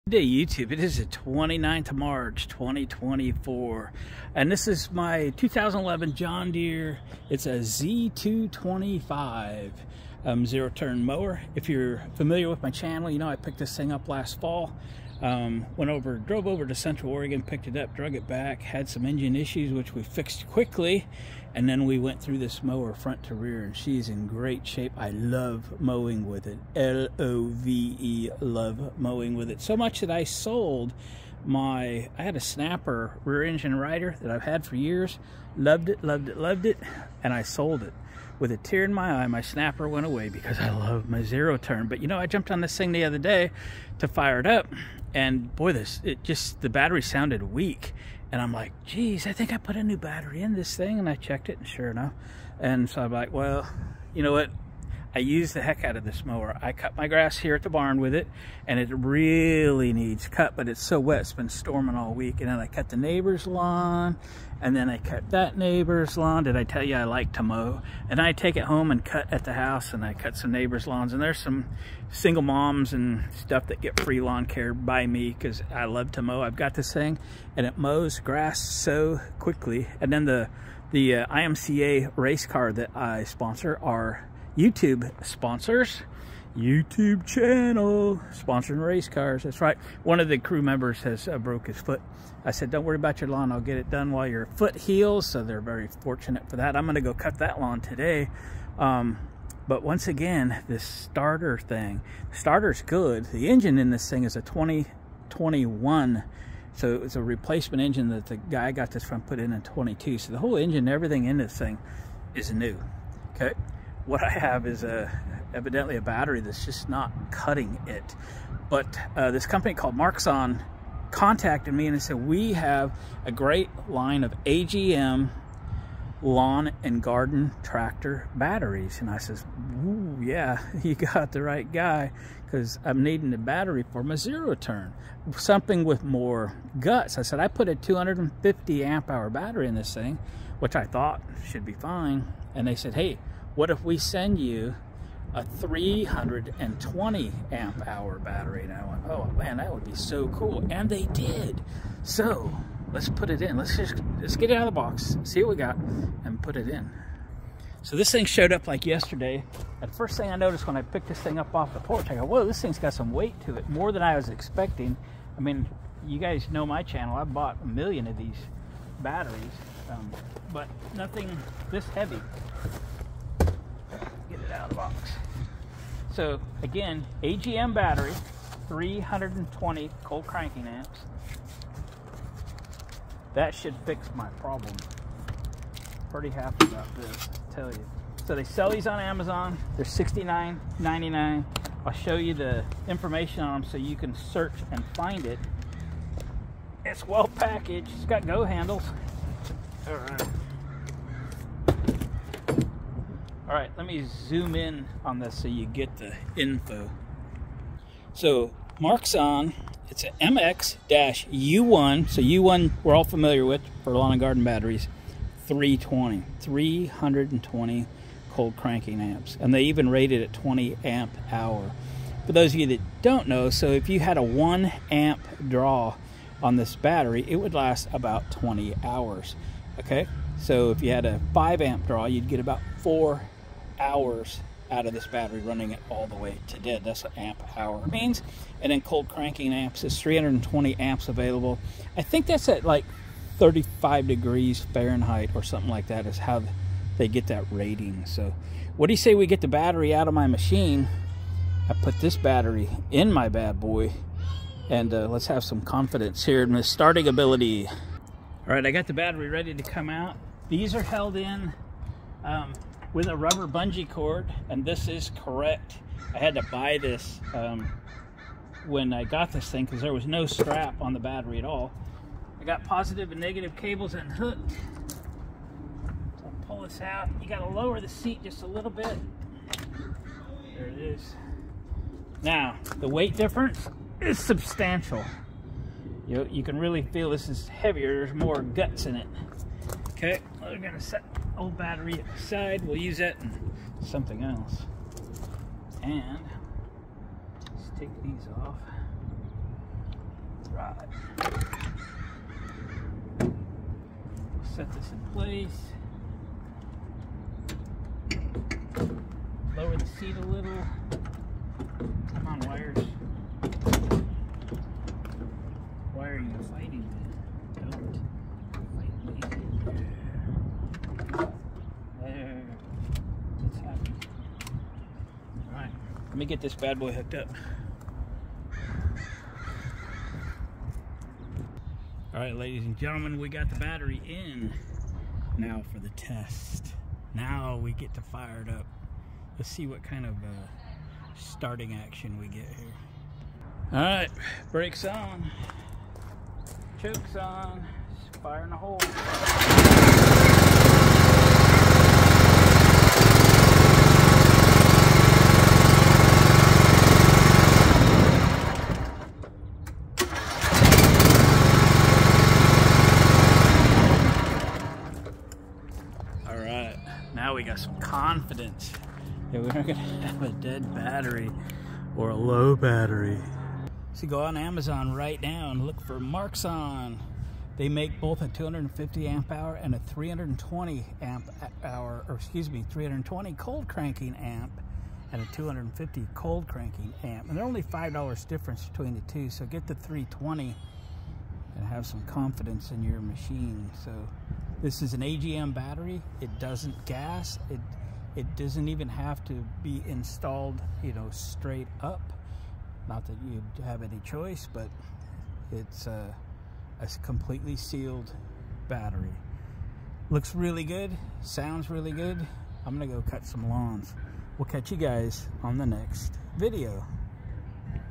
Today, I installed a new Marxon AGM, 320 CCA Battery in my John Deere Zero Turn Mower!
Marxon U1-320

Marxon U1-250